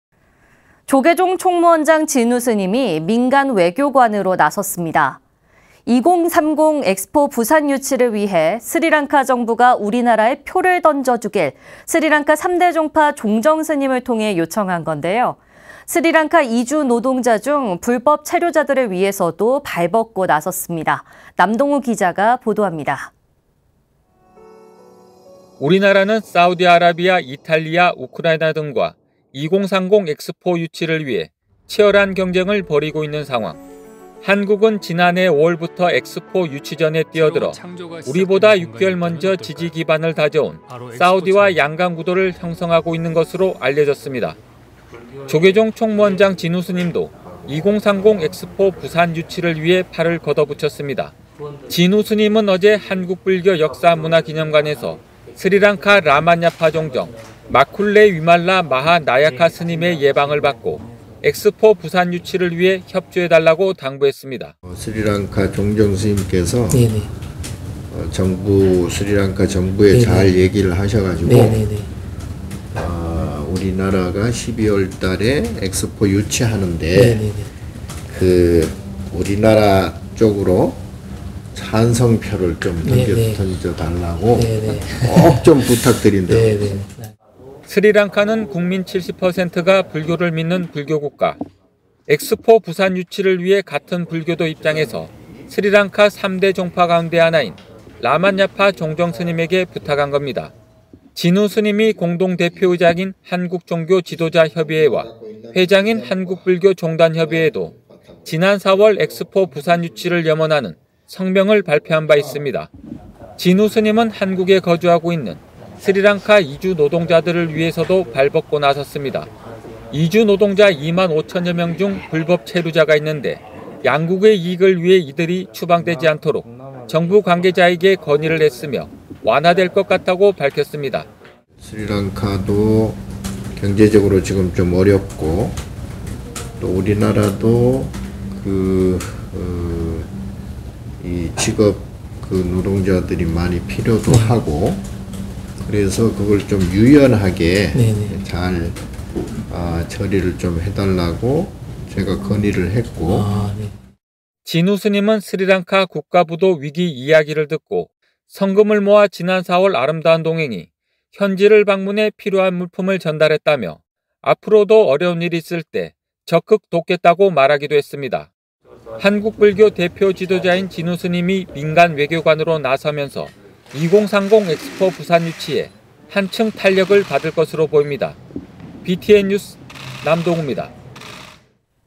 [BTN뉴스] 2030엑스포 부산 유치 진우스님도 나섰다
[앵커]

조계종 총무원장 진우스님이 민간외교관으로 나섰습니다. 2030 엑스포 부산 유치를 위해 스리랑카 정부가 우리나라에 표를 던져주길 스리랑카 3대 종파 종정 스님을 통해 요청한 건데요, 스리랑카 이주노동자 중 불법체류자들을 위해서도 발 벗고 나섰습니다. 남동우 기자가 보도합니다.
[리포트]

우리나라는 사우디아라비아, 이탈리아, 우크라이나 등과 ‘2030 엑스포’ 유치를 위해 치열한 경쟁을 벌이고 있는 상황.

한국은 지난해 5월부터 엑스포 유치전에 뛰어들어 우리보다 6개월 먼저 지지 기반을 다져온 사우디와 ‘양강 구도’를 형성하고 있는 것으로 알려졌습니다.

조계종 총무원장 진우스님도 2030 엑스포 부산 유치를 위해 팔을 걷어붙였습니다.

진우스님은 어제 한국불교역사문화기념관에서 스리랑카 라만야파 종정 마쿨레 위말라 마하 나야카스님의 예방을 받고 엑스포 부산 유치를 위해 협조해달라고 당부했습니다.

진우스님/조계종 총무원장
(스리랑카 종정 스님께서 스리랑카 정부에 잘 얘기를 하셔가지고 우리나라가 12월 달에 엑스포 유치하는데 우리나라 쪽으로 찬성표를 던져달라고 꼭 좀 부탁드린다고...)

스리랑카는 국민 70퍼센트가 불교를 믿는 불교국가.

엑스포 부산 유치를 위해 같은 불교도 입장에서 스리랑카 3대 종파 가운데 하나인 라만야파 종정 스님에게 부탁한 겁니다.

진우스님이 공동대표의장인 한국종교지도자협의회와 회장인 한국불교종단협의회도 지난 4월 엑스포 부산 유치를 염원하는 성명을 발표한 바 있습니다.

진우스님은 한국에 거주하고 있는 스리랑카 이주노동자들을 위해서도 발 벗고 나섰습니다.

이주노동자 2만 5천여 명 중 불법체류자가 있는데 양국의 이익을 위해 이들이 추방되지 않도록 정부 관계자에게 건의를 했으며 완화될 것 같다고 밝혔습니다.

(스리랑카도 경제적으로 지금 어렵고 또 우리나라도 직업 노동자들이 많이 필요하고 그래서 그걸 유연하게 잘 처리를 해달라고 건의를 했고...)

진우스님은 스리랑카 국가부도 위기 이야기를 듣고 성금을 모아 지난 4월 아름다운동행이 현지를 방문해 필요한 물품을 전달했다며 앞으로도 어려운 일이 있을 때 적극 돕겠다고 말하기도 했습니다.

한국불교 대표 지도자인 진우스님이 민간외교관으로 나서면서 2030 엑스포 부산 유치에 한층 탄력을 받을 것으로 보입니다.

BTN 뉴스 남동우입니다.